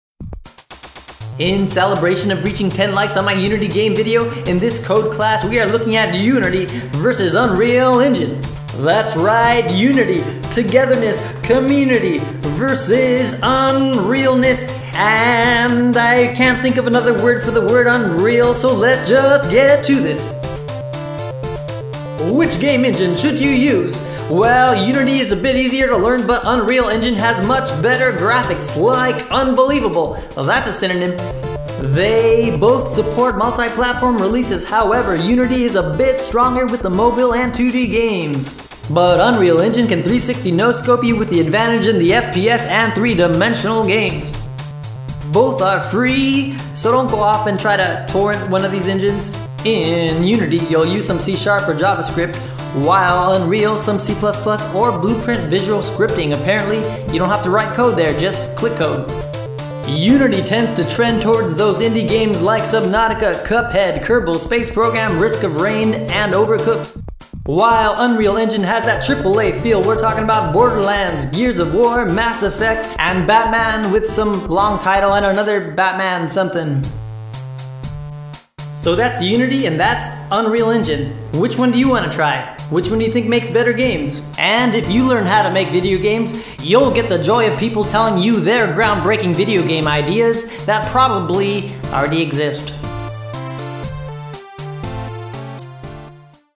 UNITY vs UNREAL ENGINE!
A quick comparison between the popular game engines Unity and Unreal Engine. Which one do you prefer?